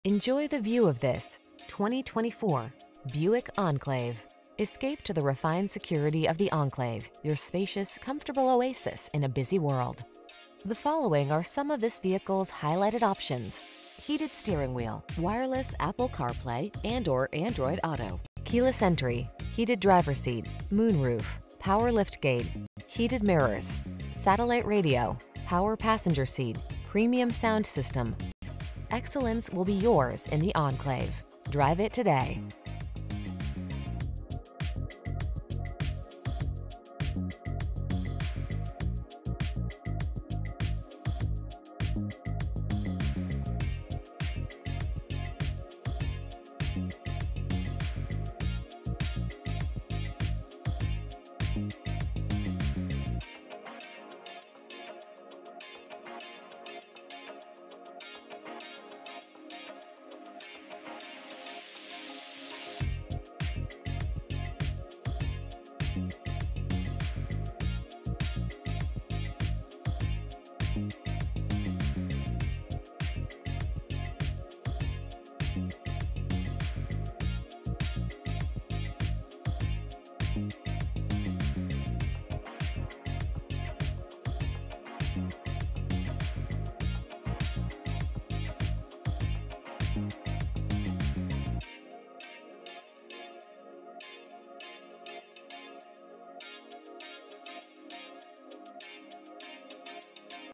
2024 Buick Enclave Essence Naperville, Plainfield, Aurora, Oswego, Chicago IL
New 2024 Buick Enclave Essence available at Woody Buick located in 1585 W Ogden Ave, Naperville, IL, 60540.
Stock#: B7297 - VIN#: 5GAEVAKW0RJ119819

Enjoy the view of this   2024  Buick Enclave. Escape to the refined security of the Enclave, your spacious, comfortable oasis in a busy world.  The following are some of this vehicles highlighted options: Heated Steering Wheel
, Wireless Apple Carplay®/Android Auto®, Keyless Entry, Heated Driver Seat, Moonroof, Power Liftgate, Heated Mirrors, Satellite Radio, Power Passenger Seat, Premium Sound System. Excellence will be yours in the Enclave. Drive it today.